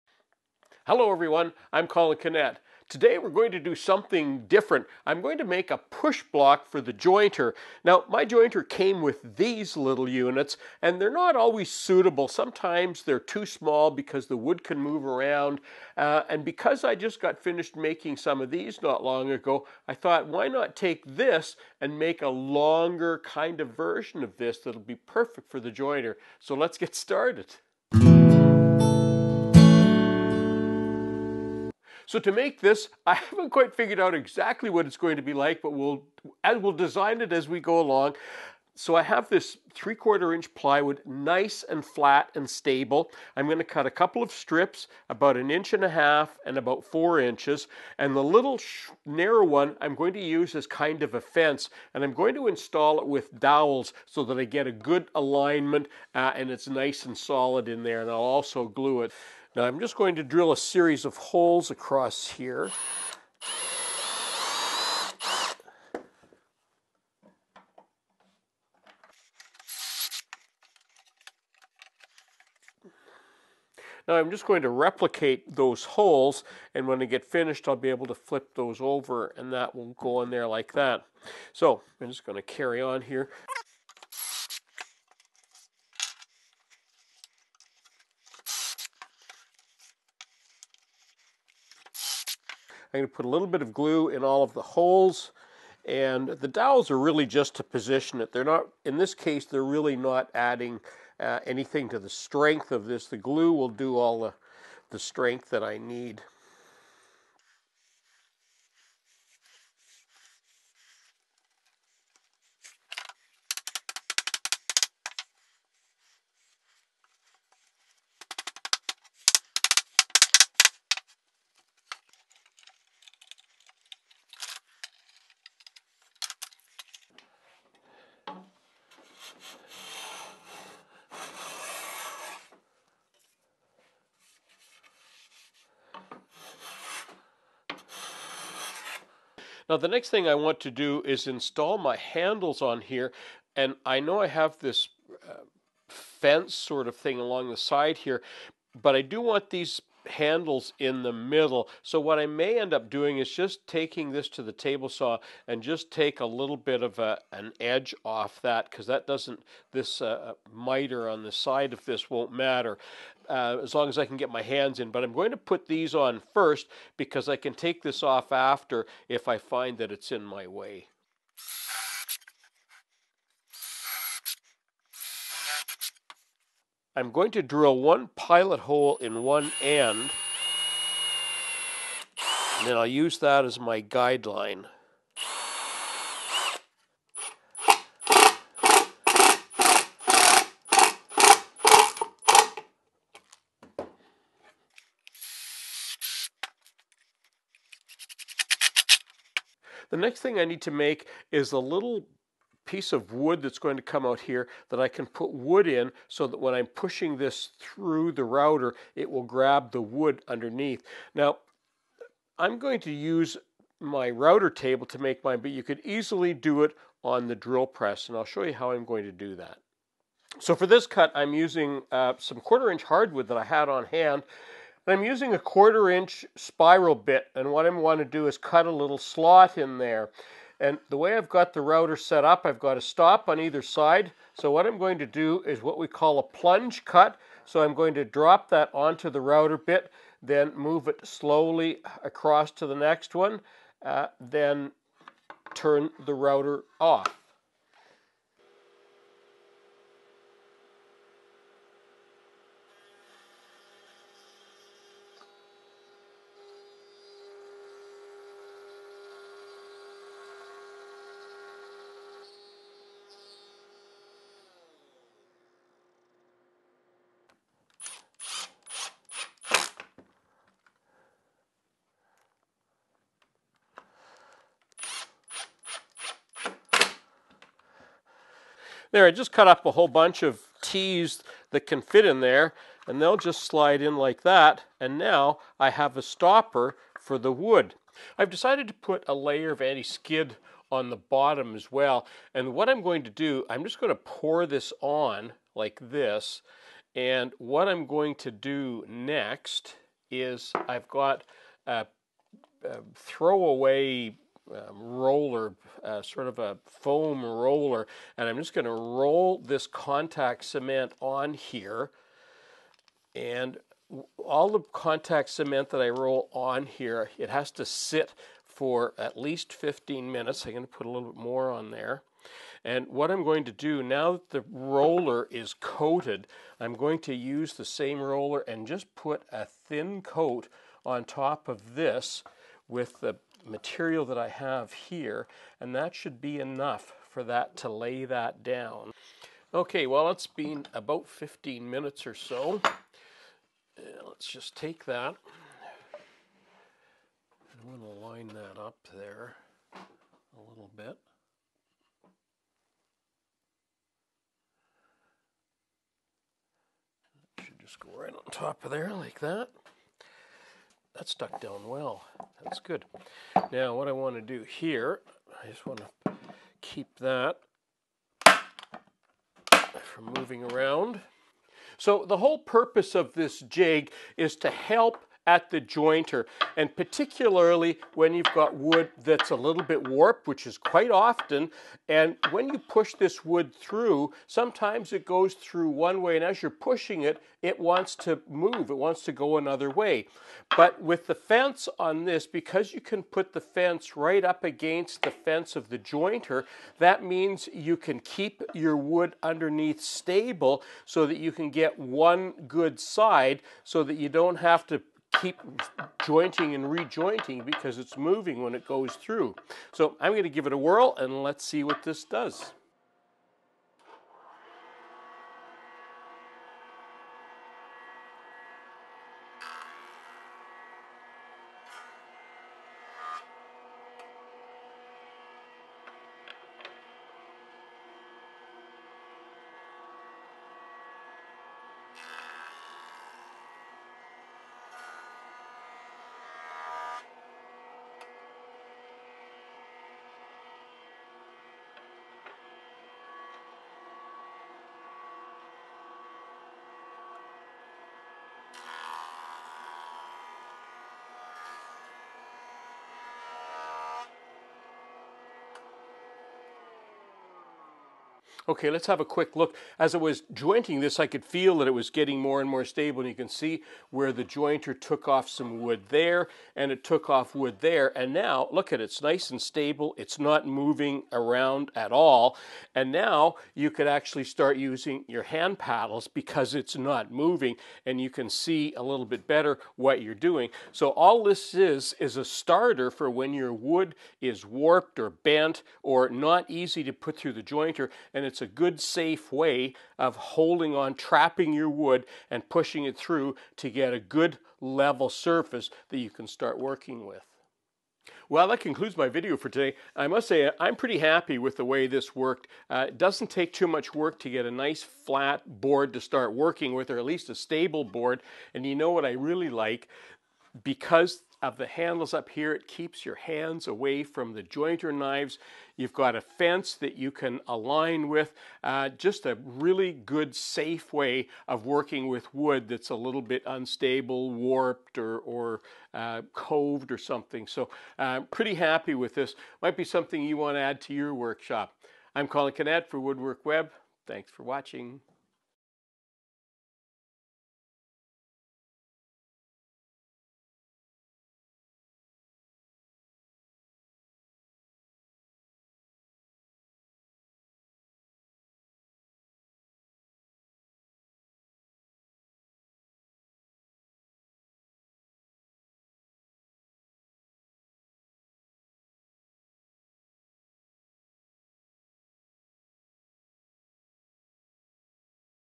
Make an Edge Jointing Sled Jig / Edge Jointer Safety Push Block
In this video, we show you how to make an edge jointing sled jig / edge jointer safety push block.

The purpose of the push block sled jig is to be able to safely handle smaller pieces of wood that are warped, and run them through the edge jointer until I can get an edge to them that is stable enough to use my hand paddles to continue to work the wood as needed.